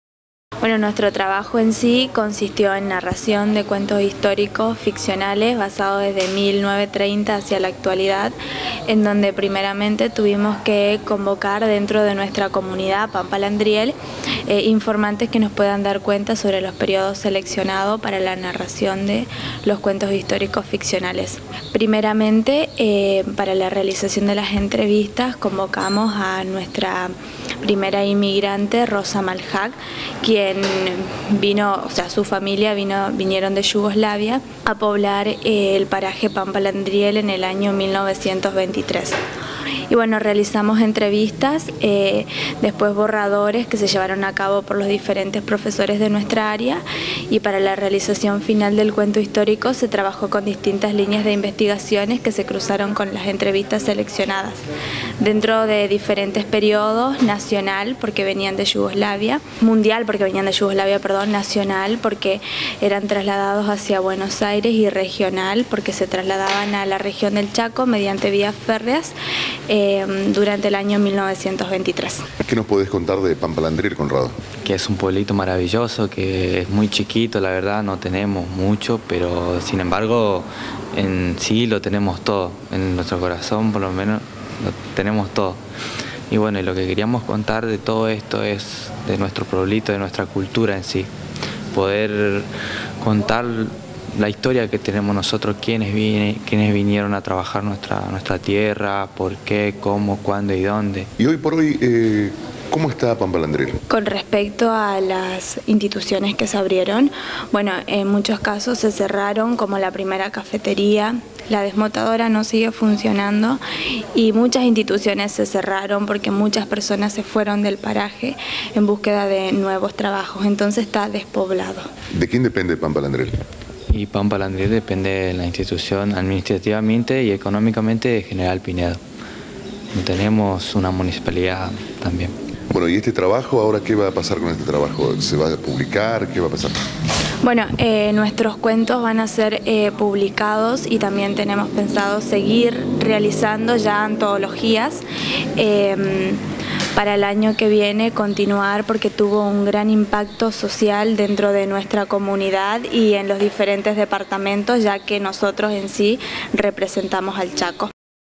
C9 - CHACO; DESTACADOS EN TECNÓPOLIS
DOS ALUMNOS DE PAMPA LANDRIEL  FUERON PREMIADOS
ACTUACIÓN DESTACADA DE DOS ALUMNOS EN TECNÓPOLIS
Ludmila Ledesma y Conrado Iguich son alumnos de 4to año de la Escuela Secundaria N° 120 de Pampa Landriel.  Estos jóvenes estudiantes tuvieron una actuación destacada en la Feria Nacional de Innovación Educativa que se desarrolló  en el predio de Tecnópolis logrando el primer puesto en la categoría Ciencias Sociales y Lengua con su trabajo «Redacción de Cuentos históricos» .N9 entrevisto a estos jovenes orgullosos de su paraje quienes sueñan con un museo para su comunidad-
LUDMILA LEDESMA // CONRRADO IGUICH